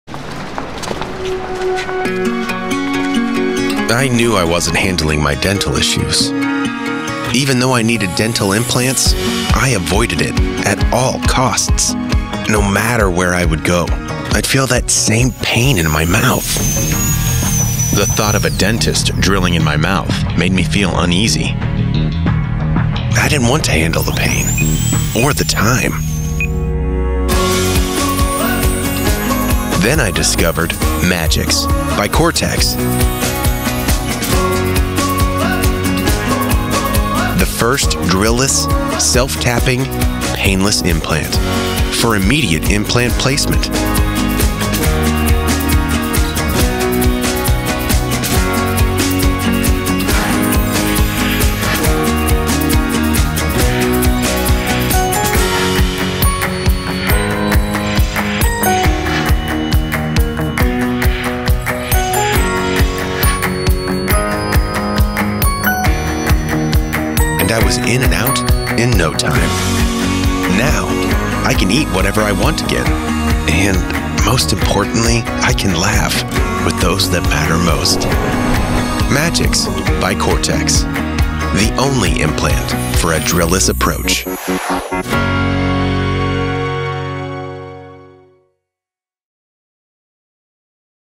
Magix Implant by Cortex - For The Dentist And The Patient
Cortex innovative implant - Magix.

Implant for a drill-less approach.

Bone preserving
Drill-less
Pain-less
Time-saving